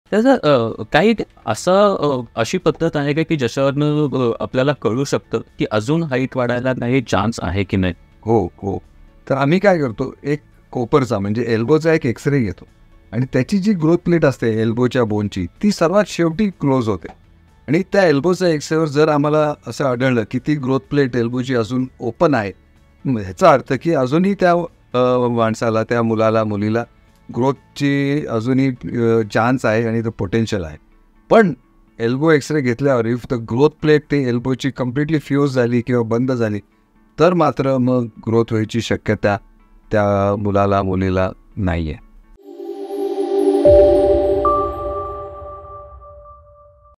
अशी कोणती पद्धत आहे ज्यातून समजेल की अजून उंची वाढू शकते? । Dr. Parag Sancheti
M2 Did you like what you saw?